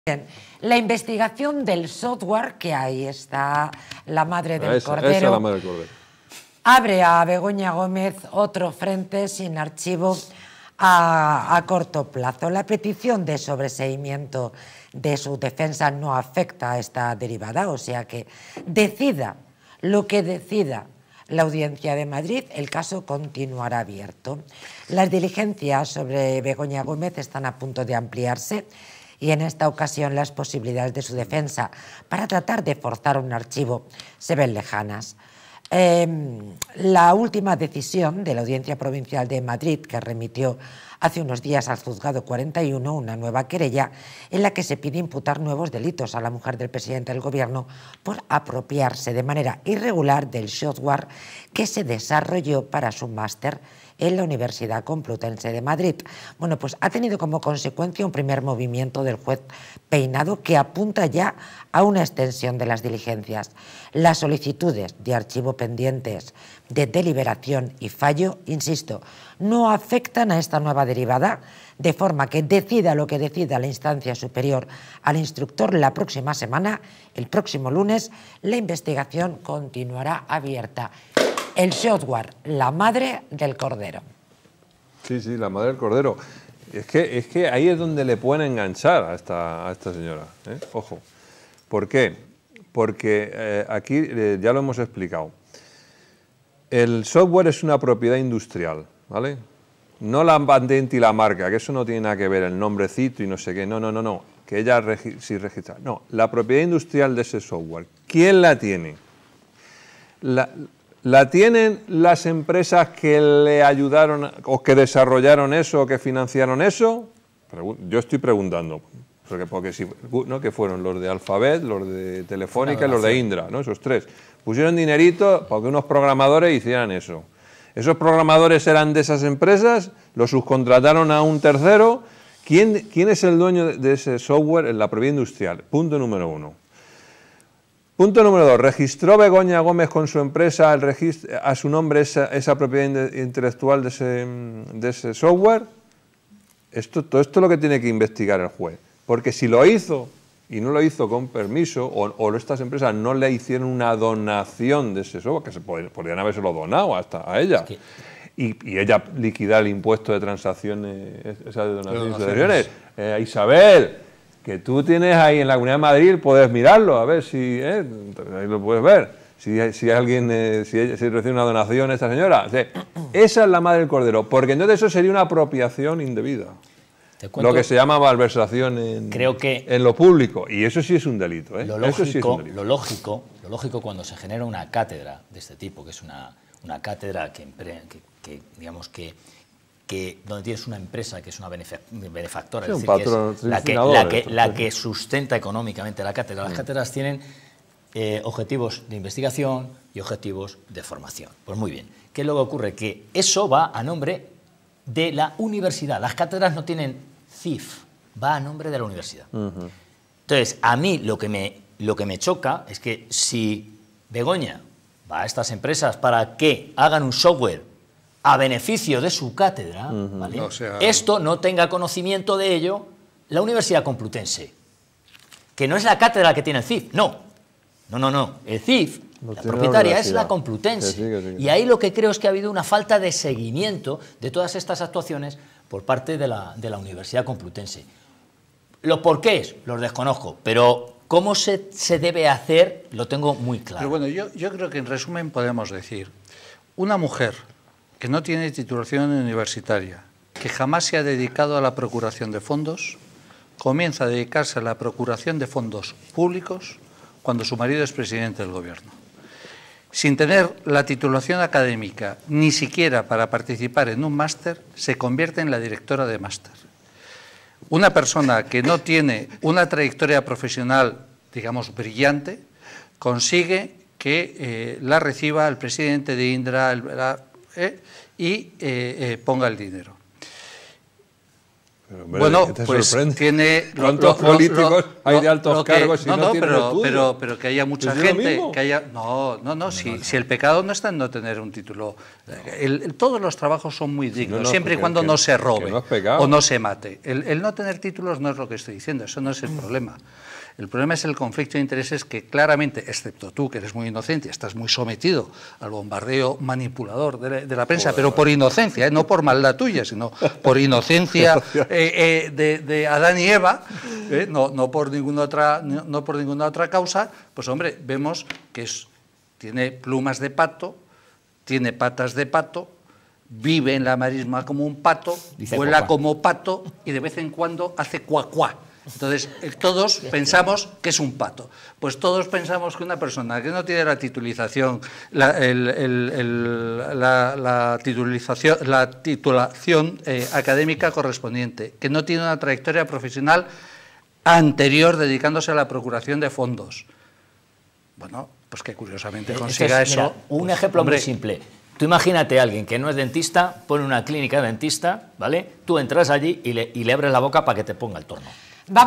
🔴La investigación del 'software' abre a Begoña Gómez otro frente sin archivo a corto plazo🔴
En este video, abordaremos la investigación del 'software' que enfrenta Begoña Gómez, sin archivo a corto plazo. ¡No te lo pierdas en Distrito TV!distritotv